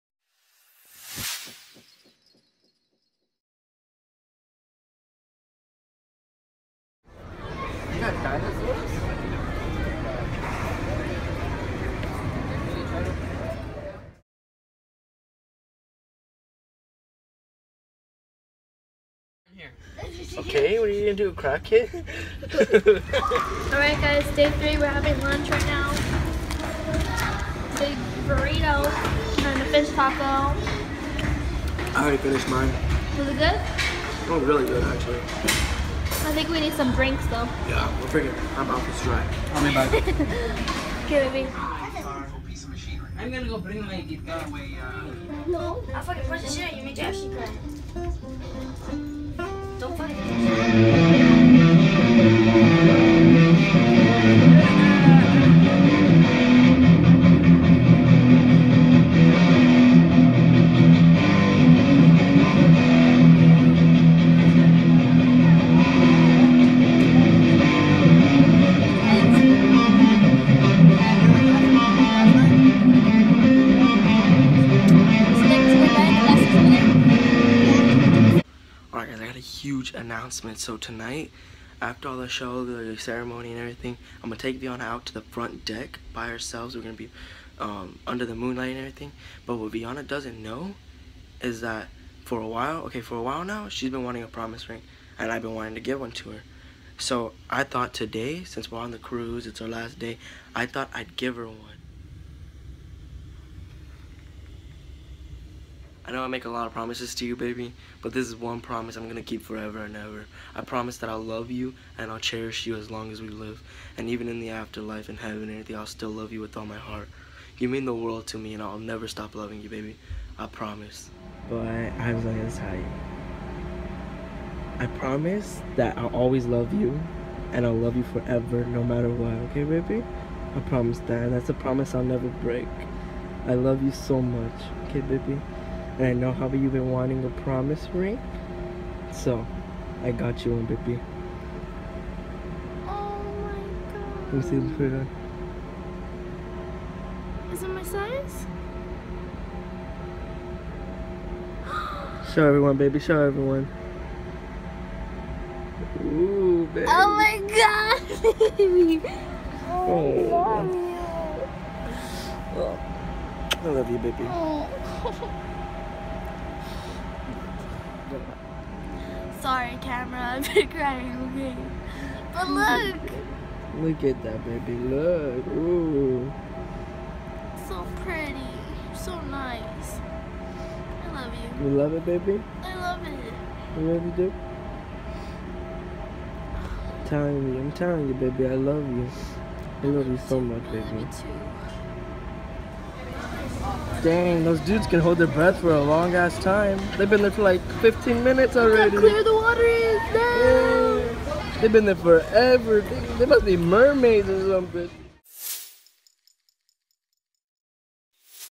DAY 3: HUGE SURPRISE 😱|| VLOG#5
Surprised Vee with a promise ring 💍

⚠️GO CHECK OUT DAY 1&2⚠️